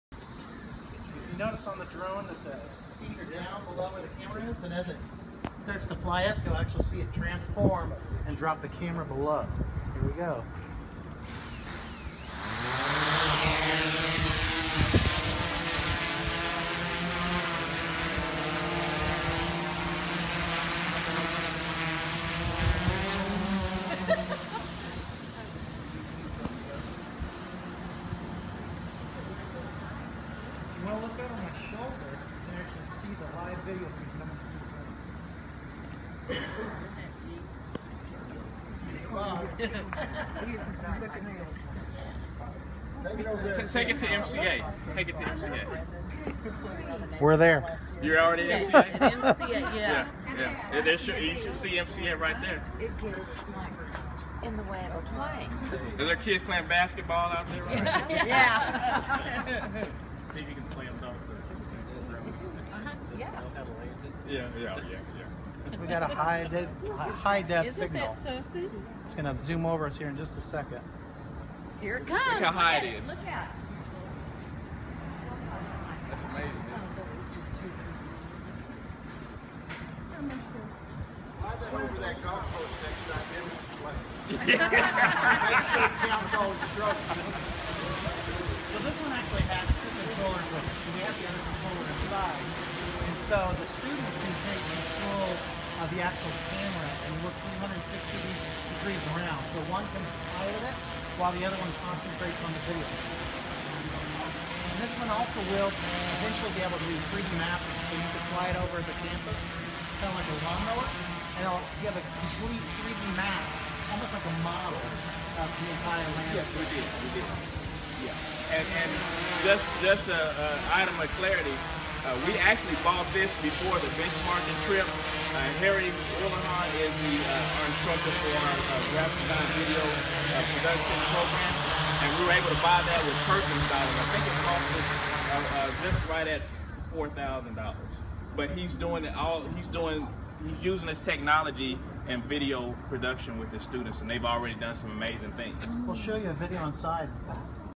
It's a bird? No... It's a Plane? No.... It is the first Metro Tech video drone!
Harry Wolohon from the Metro Tech Digital Video and Web program demonstrates our new DJI Inspire 1 video drone to the Metro Tech Board of Education.  Harry's students are learning how to shoot and edit video and this is just one of the cutting edge tools they are using in the classroom.